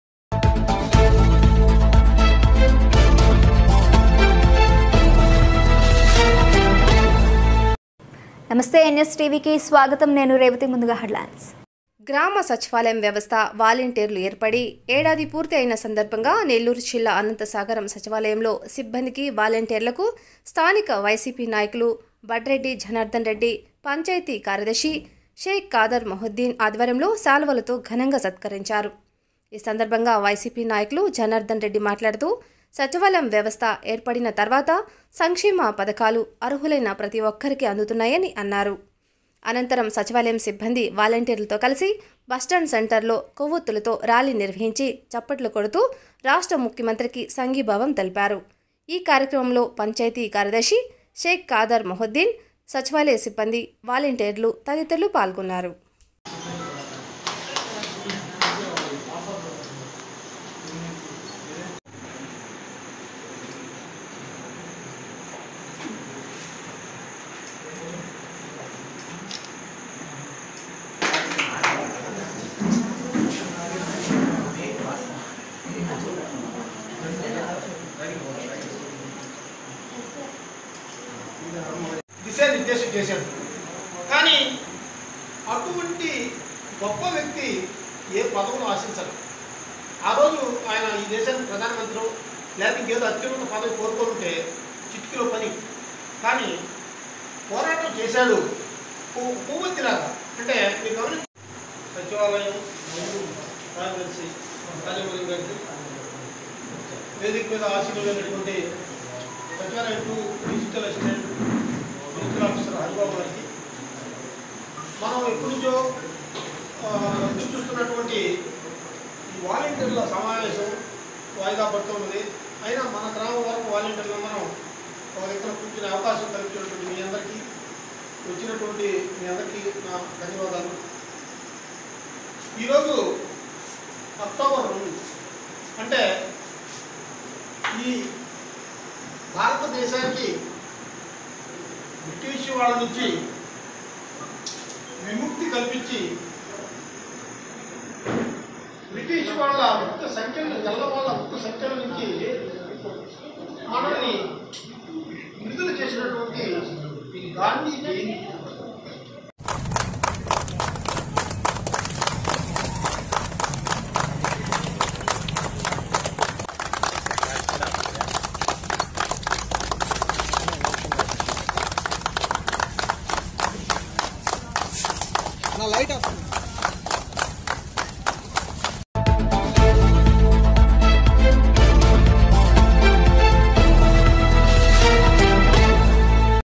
Nstv#సచివాలయ వ్యవస్థ ఏడాది పూర్తి చేసుకున్న సందర్భంగా వాలంటీర్లకు సన్మానం
Nstv#నెల్లూరు జిల్లా,ఆత్మకూరు||సచివాలయ వ్యవస్థ ఏడాది పూర్తి చేసుకున్న సందర్భంగా వాలంటీర్లకు సన్మానం